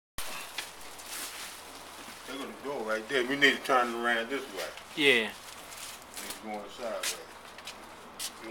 Victor Safe Company safe: Home at Last...!!!
This is a closer view of the safe that was being offloaded. It is very solid. We do not have the combination, however, the door is open so I guess I will take off the four screws inside the door and try to find out the combination...